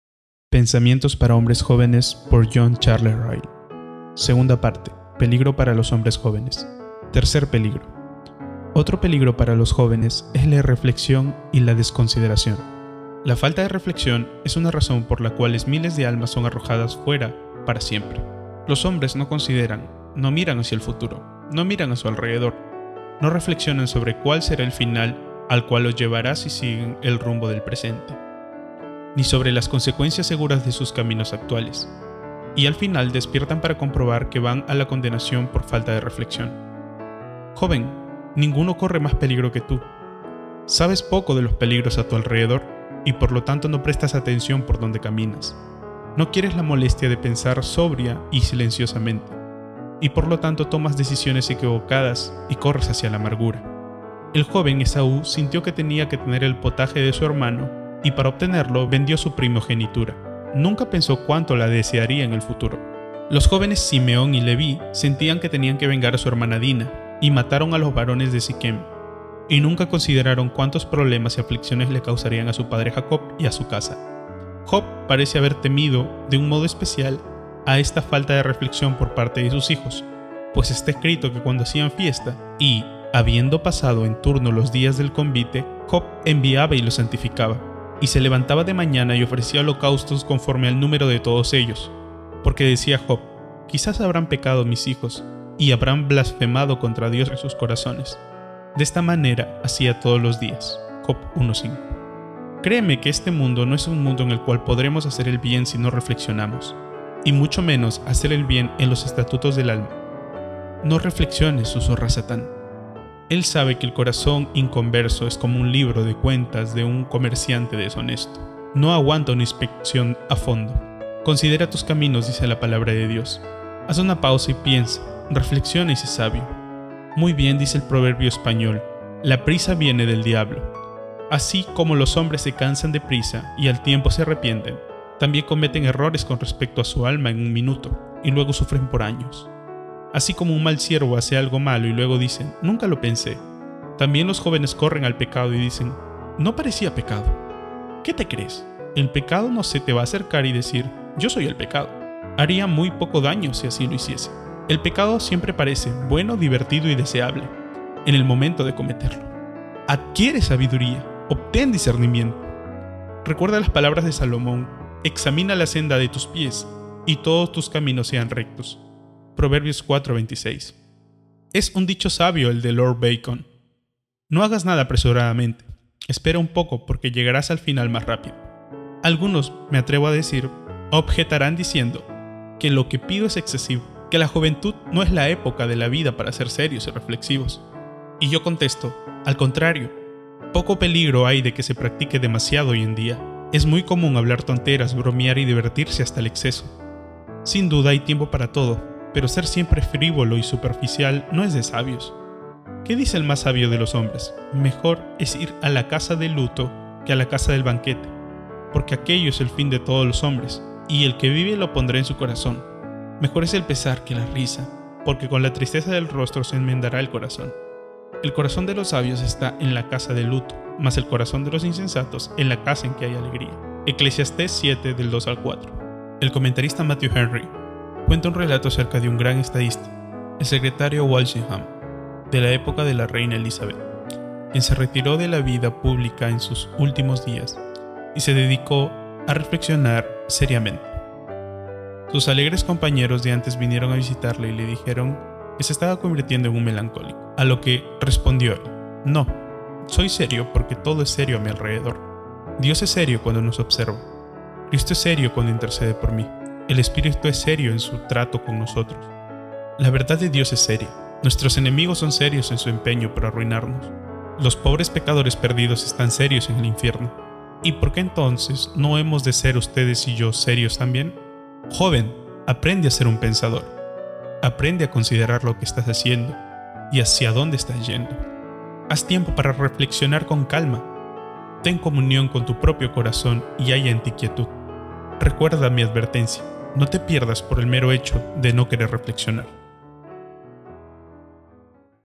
FALTA DE REFLEXIÓN - Peligros | Pensamientos Para Jóvenes - J.C. Ryle (Audiolibro Cristiano)
Yo mismo me estoy haciendo viejo, pero hay unas cuantas cosas que recuerdo muy bien de mi juventud. Recuerdo vívidamente los gozos, temores, las tristezas, esperanzas, tentaciones y dificultades, las decisions equivocadas y los sentimientos mal fundados, los errores y las aspiraciones que rodean y acompañan la vida del joven. Si puedo decir algo para mantener a algún joven en el camino correcto y protegerlo de las faltas y los pecados, los cuales pueden dañar sus perspectivas en el tiempo y la eternidad, estaré muy agradecido.

Me propongo hacer cuatro cosas:
1. Mencionaré algunas razones generales por las cuales los jóvenes necesitan ser exhortados.
2. Haré notar algunos peligros especiales contra los cuales los jóvenes necesitan ser advertidos.
3. Daré algunos consejos generales que ruego a los jóvenes reciban.
4. Y estableceré algunas reglas de conducta especiales que aconsejo encarecidamente a los jóvenes seguir.

En cada uno de estos cuatro puntos, tengo algo que decir, y oro a Dios que lo que diga sea de bien para algún alma.

John Charles Ryle (10 de mayo de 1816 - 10 de junio de 1900) fue un obispo anglicano evangélico inglés. Fue el primer obispo anglicano de Liverpool .